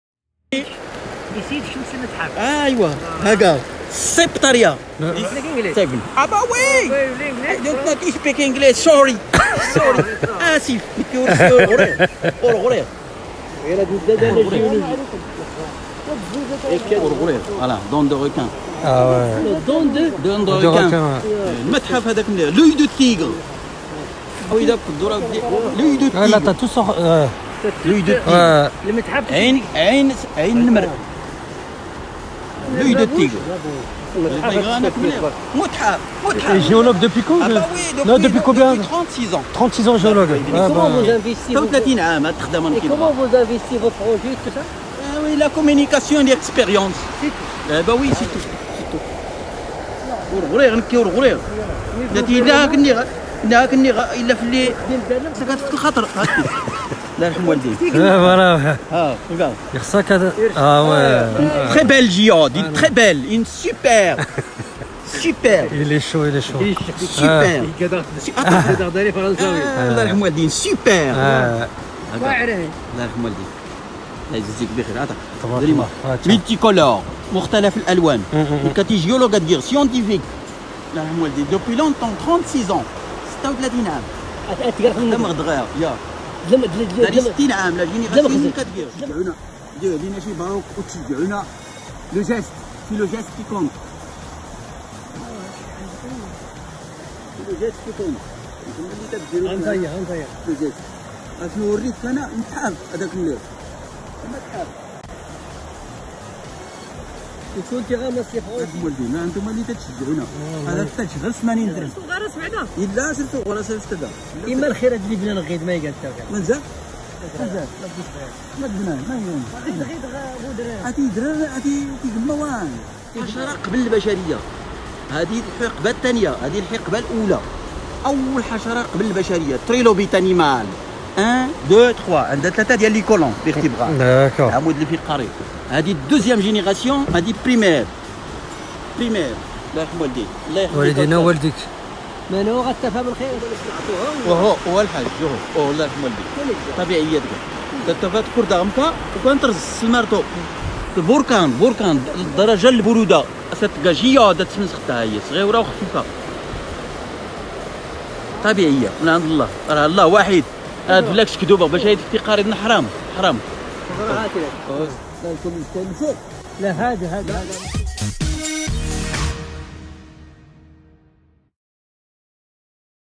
2022 TRAVEL WORLD RENCONTRE AVEC HASSAN GÉOLOGUE DE PIERRE QWARTZ OURIKA VALLEY MAROC 🇲🇦
Quelles sont les vertus du quartz ?
Le quartz permettrait aussi d'augmenter toutes les propriétés des autres pierres. Ce serait un bon équilibreur de diabète et de sclérose multiple. Il soulagerait des migraines, des vertiges et les étourdissements. Il apaiserait les zones douloureuses, en particulier en cas de brulure et d'ampoules.